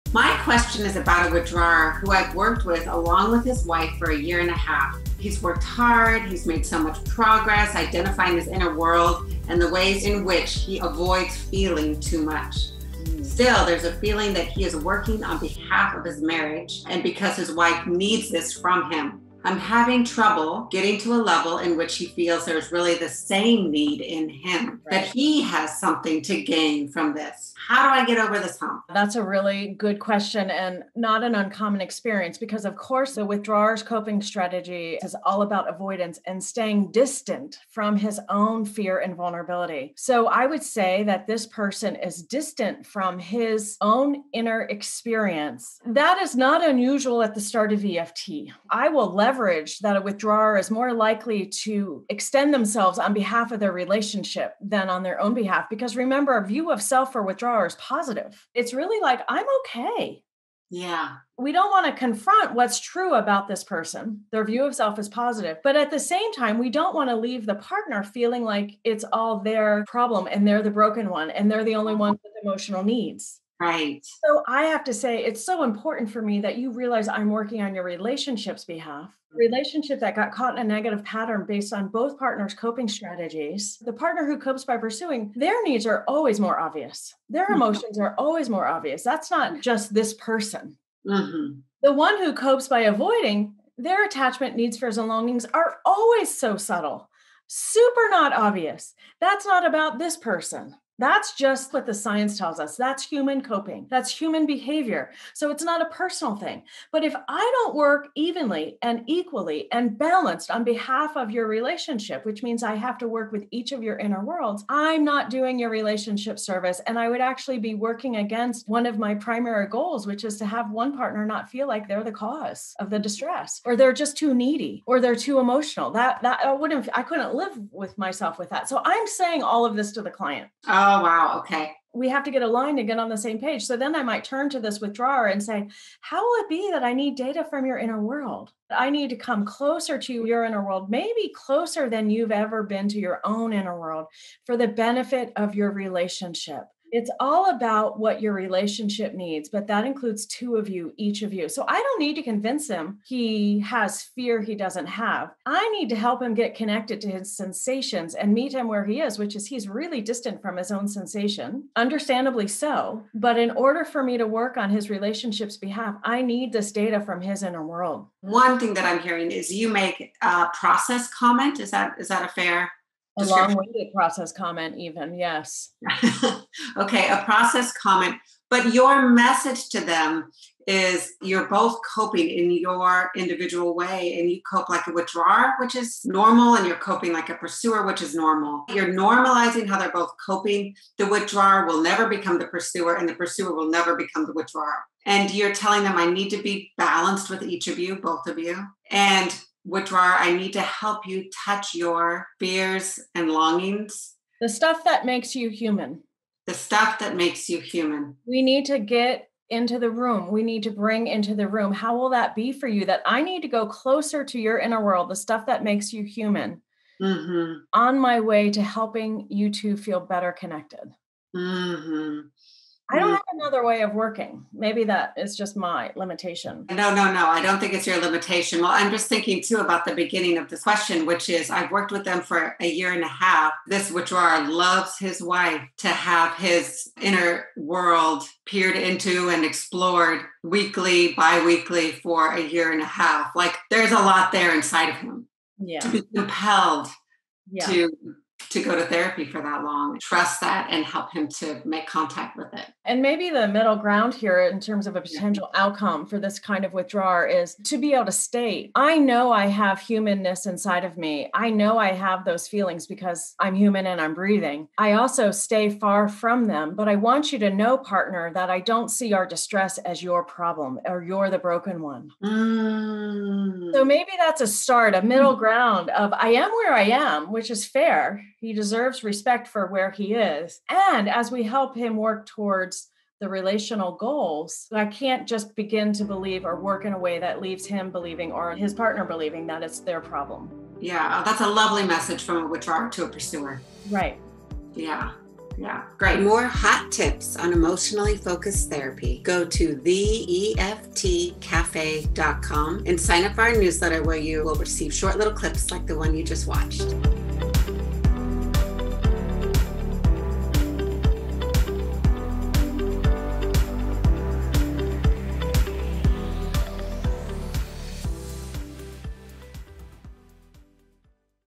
Dealing With A Distant Withdrawer in EFT
Registration for the EFT Cafe opens February 14!

Kathryn Rheem and Jennifer Olden discuss the classic situation in which a withdrawer is distant in EFT session.